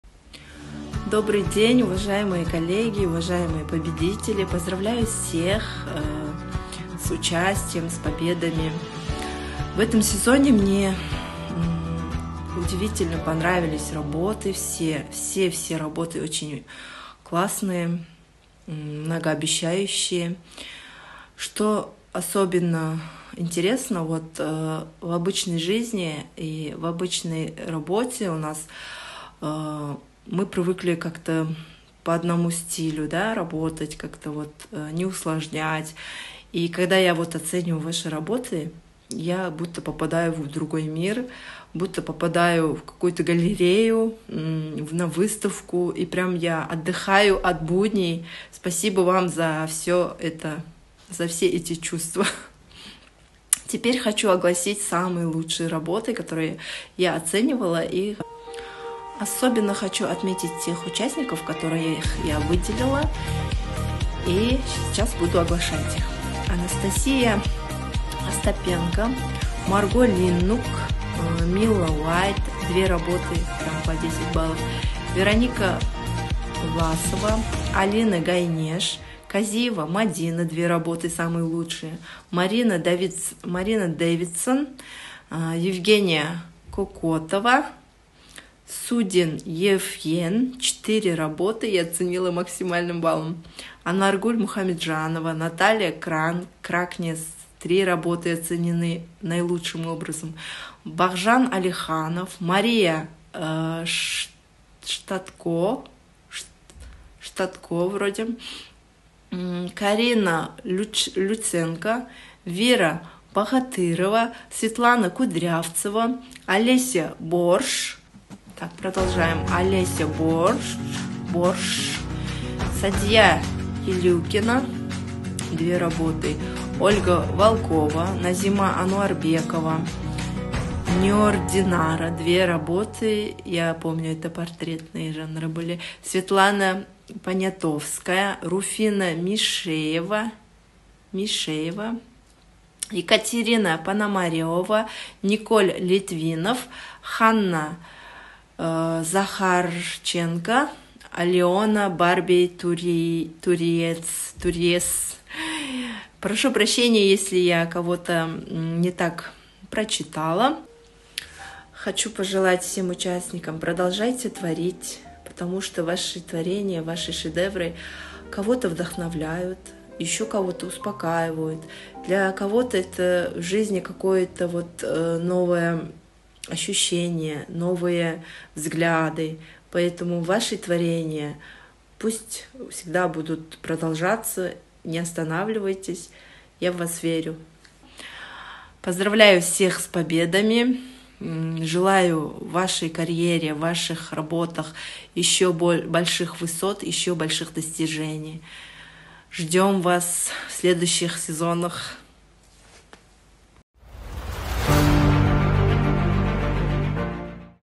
Video evaluation of season 60 - Golden Time Talent jury - Kushekbayeva Lyazzat 🇰🇿
Dear friends!
We publish a video assessment of the 60 season from the jury of the Golden Time Talent contest - Kushekbayeva Lyazzat 🇰🇿